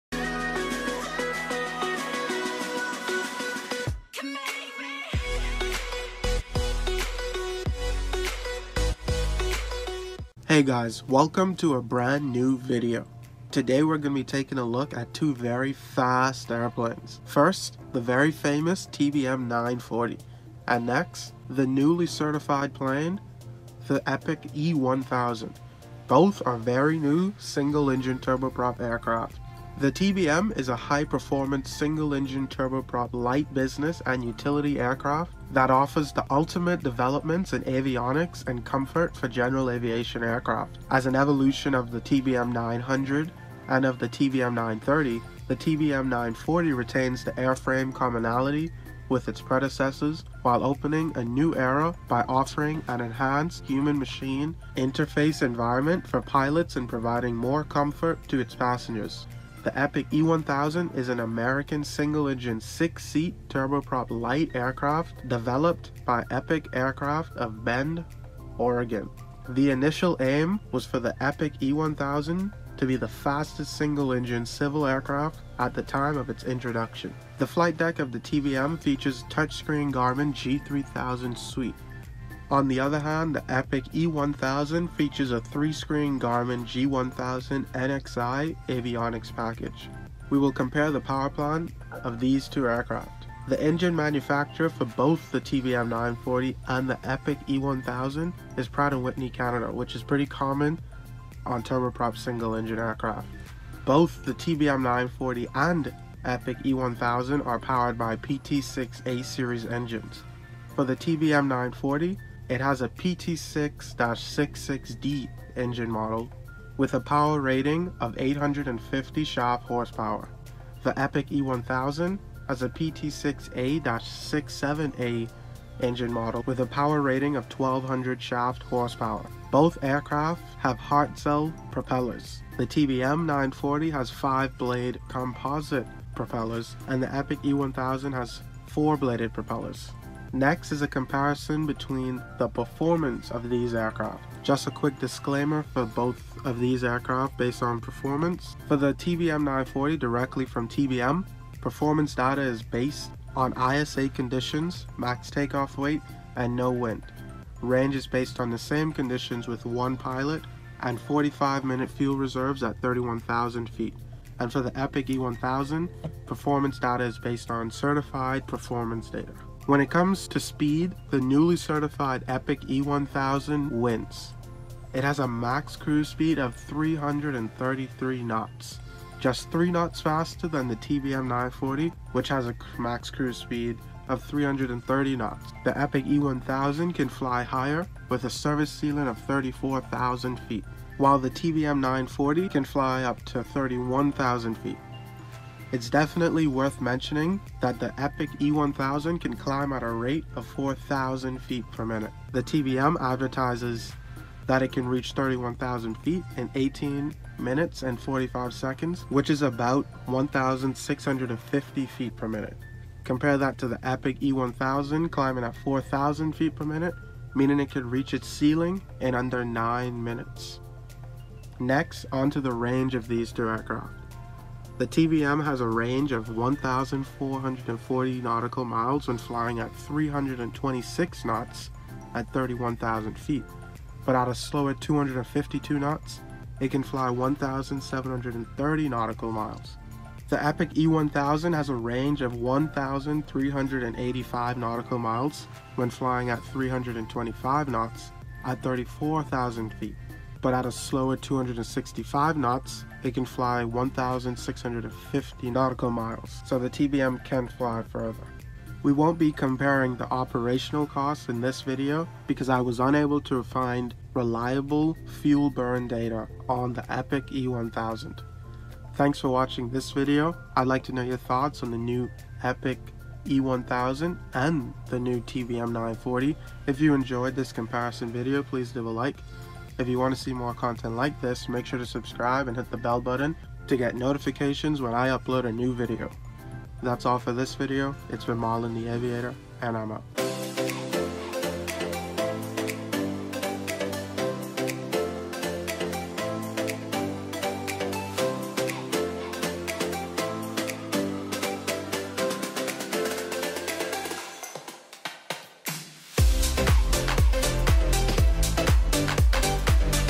Better Aircraft - Epic E1000 vs TBM 940
Today we will be taking a look at two very fast airplanes.
First the very famous TBM 940 and the next is a newly certified plane the Epic E1000.
Both are very new single-engine turboprop aircraft.

The  TBM is a high performance single-engine turboprop light business and utility aircraft that offers the ultimate developments in avionics and comfort for a general aviation aircraft. As  an  evolution  of  the  TBM  900  and  of  the  TBM  930, the TBM 940 retains the airframe commonality with its predecessors while  opening  a  new  era  by offering  an  enhanced  human-machine  interface environment for pilots and providing more comfort  to  its  passengers.

The Epic E1000 is an American single-engine, six-seat, turboprop light aircraft under
development by Epic Aircraft of Bend, Oregonaim is that the E1000 will be the fastest
single-engine civil aircraft at its time of introduction